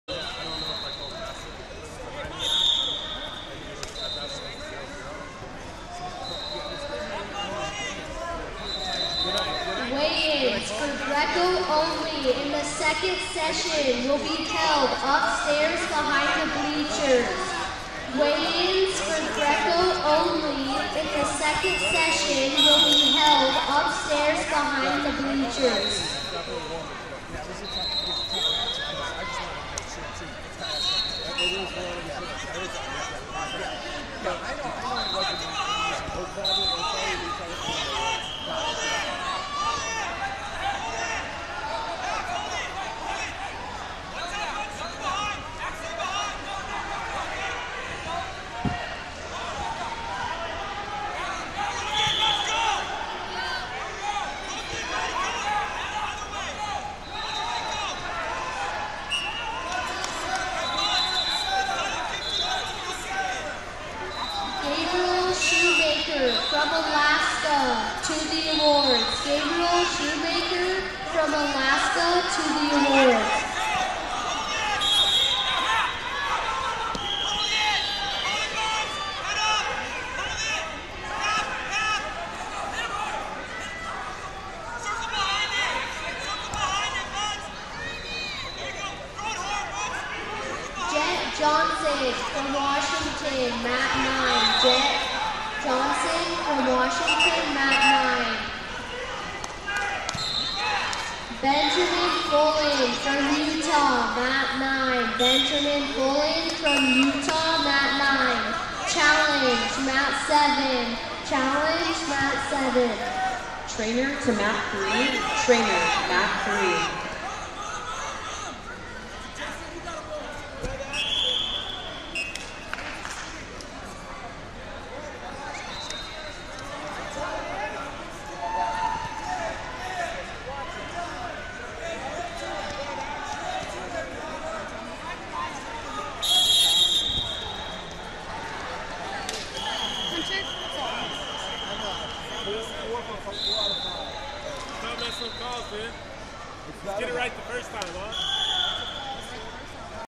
53 Lbs Round 3 - Uriah Doyle, CA Vs Aksel Polson, WY C4fa
2022 Western Regional Championships - 53 Lbs Round 3 - Uriah Doyle, CA Vs Aksel Polson, WY C4fa